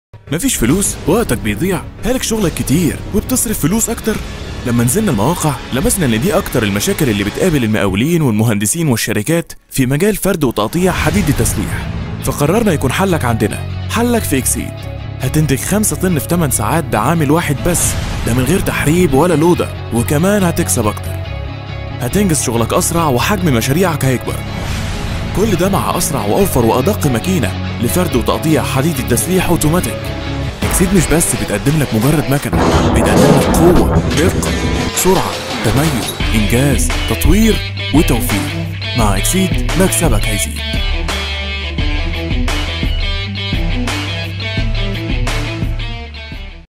مفيش فلوس !! ..طب شوف أسرع وأوفر ماكينة لفرد وتقطيع حديد التسليح في مصر
مفيش فلوس.. وقتك بيضيع.. هالك شغلك كتير،وبتصرف فلوس أكتر؟ كل المشاكل دي هتتحل مع ماكينات اكسيد✨.. اجهز واستعد لمشروعك واطلب دلوقتي عرض السعر وسيب رقمك واحنا هنكلمك ☑
كلمنا واعرف تفاصيل اكتر ع :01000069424